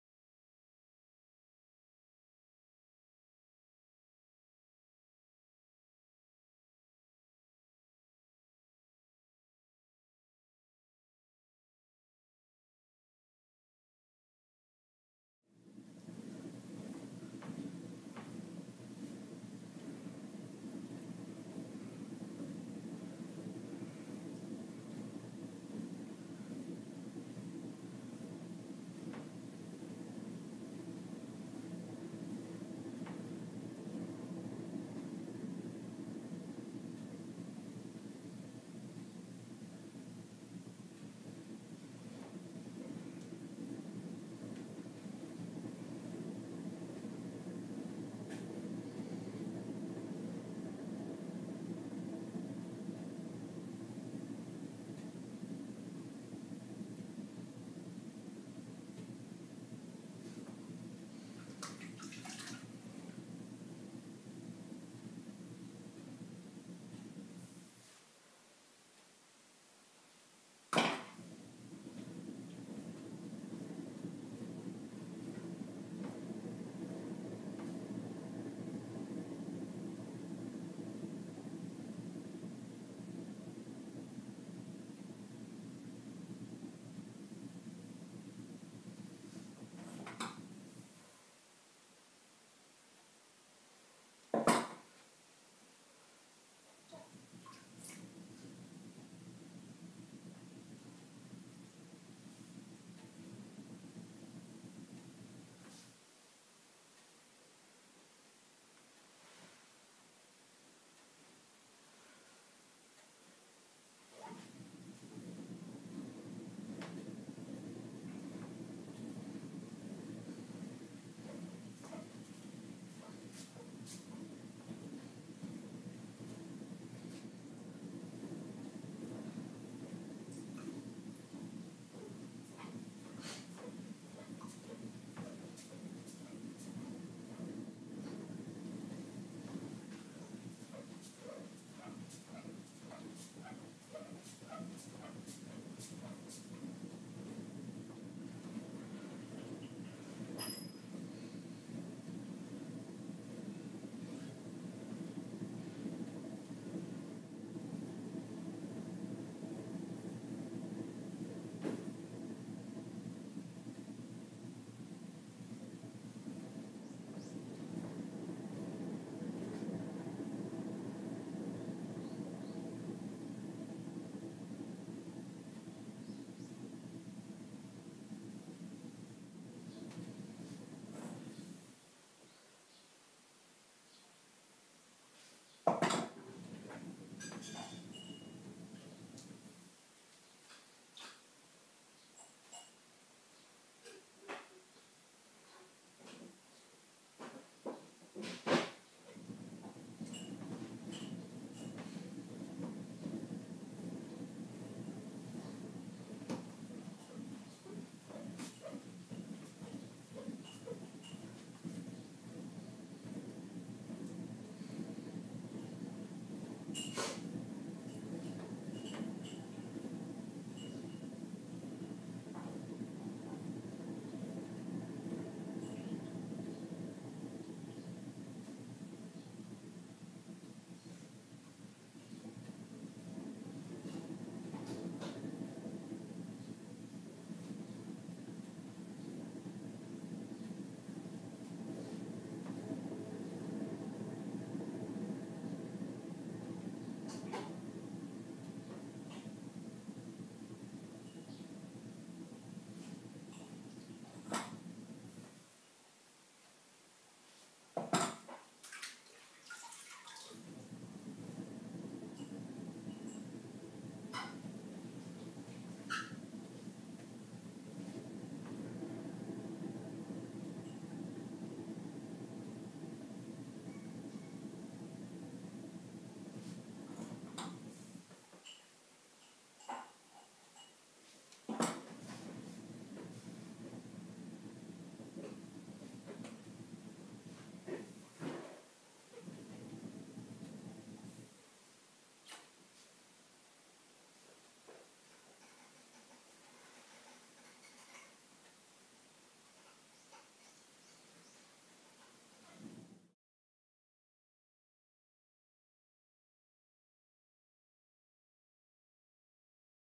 The Kick Wheel
Using the traditional Japanese Kick Wheel to throw bowls in my studio in Minakami, Japan